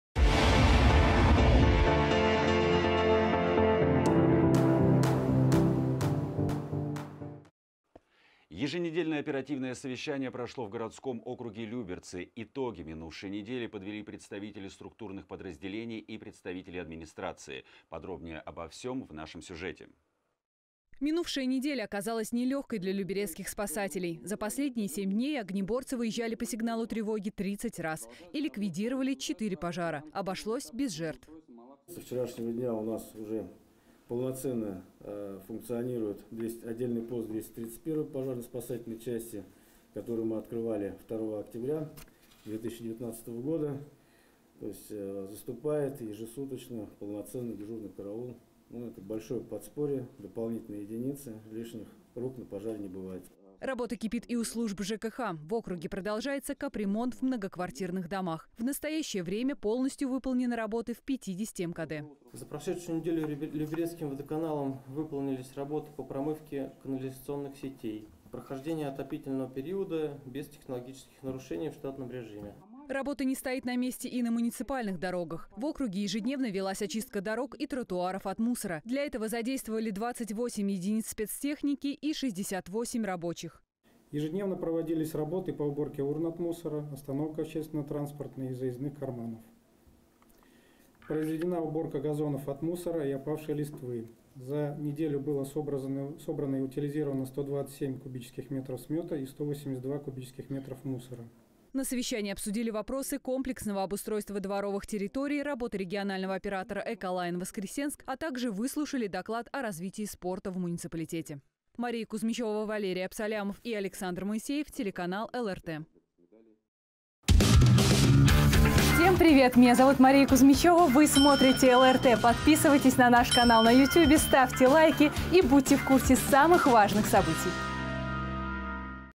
Еженедельное оперативное совещание в администрации
Еженедельное оперативное совещание прошло в городском округе Люберцы. Итоги минувшей недели подвели представители структурных подразделений и представители администрации.  Подробнее обо всем в нашем сюжете.

Минувшая неделя оказалась нелегкой для люберецких спасателей. За последние семь дней огнеборцы выезжали по сигналу тревоги 30 раз и ликвидировали 4 пожара. Обошлось без жертв.

Павел  Иванов, начальник люберецкого пожарно-спасательного гарнизона Московской области:
"Со вчерашнего дня у нас уже полноценно функционирует отдельный пост 231 пожарно-спасательной части, которую мы открывали 2 октября 2019 года.  Заступает ежесуточно  полноценный дежурный караул. Это дополнительные единицы. Лишних рук на пожаре не бывает".

Работа кипит и у служб ЖКХ. В округе продолжается капремонт в многоквартирных домах. В настоящее время полностью выполнены работы в 50 МКД.

Владимир Караваев, и.о. начальника управления ЖКХ администрации г.о. Люберцы:
"За прошедшую неделю люберецким водоканалам выполнены работы по промывке канализационных сетей. Прохождение отопительного периода без технологических нарушений в штатном режиме".

Работа не стоит на месте и на муниципальных дорогах. В округе ежедневно велась очистка дорог и тротуаров от мусора. Для этого задействовали 28 единицы спецтехники и 68 рабочих.

Валерий Подлозный, начальник управления дорожного хозяйства администрации г.о. Люберцы:
"Ежедневно проводились работы по уборке урн от мусора, остановок общественного транспорта и заездных карманов. Произведена уборка газонов от мусора и опавшей листвы. За неделю было собрано и утилизировано 127 куб.метров смета и 182 куб.метра мусора".

На совещании обсудили вопросы комплексного обустройства дворовых территорий, работы регионального оператора «Эколайн-Воскресенск», а также выслушали доклад о развитии спорта в муниципалитете.

Мария Кузмичева, Валерий Абсалямов, Александр Моисеев,